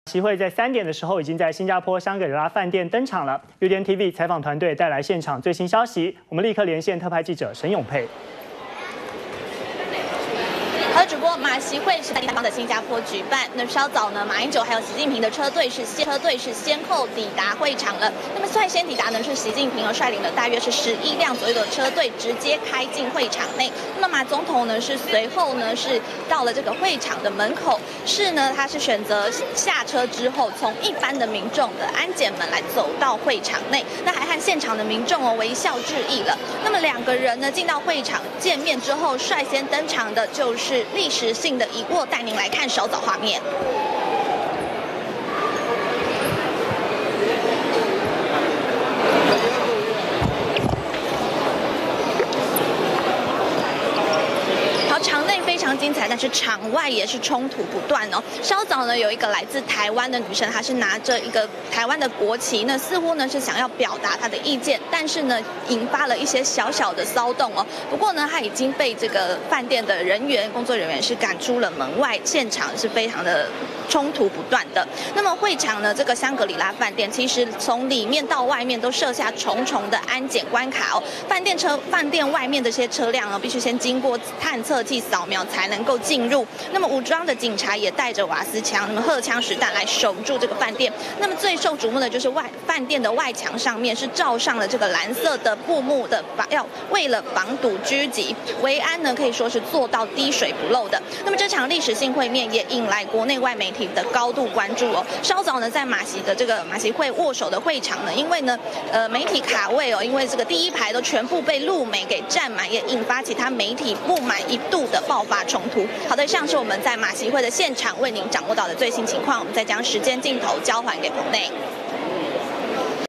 【2015.11.07】世紀之握 馬習握手歷史性一刻！ -udn tv
馬習會三點已經在新加坡香格里拉飯店登場，udn tv採訪團隊帶來現場最新消息，現場連線特派記者沈詠珮。完整內容 , FB , Twitter , G+